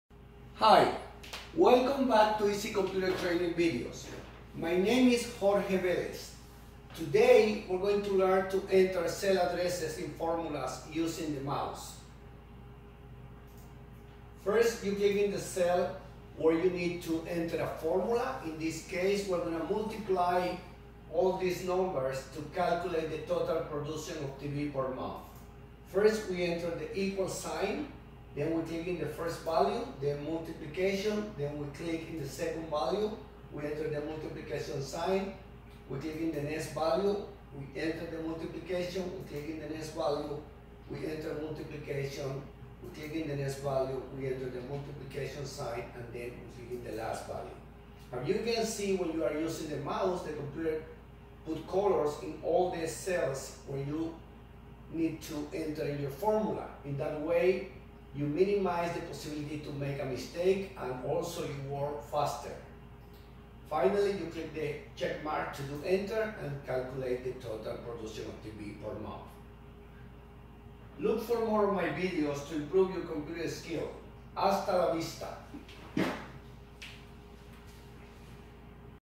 Microsoft Excel - Entering Formulas Using the Mouse.
Using the Mouse to enter cell addresses in a Formula. Work faster and secure when creating formula in Microsoft Excel!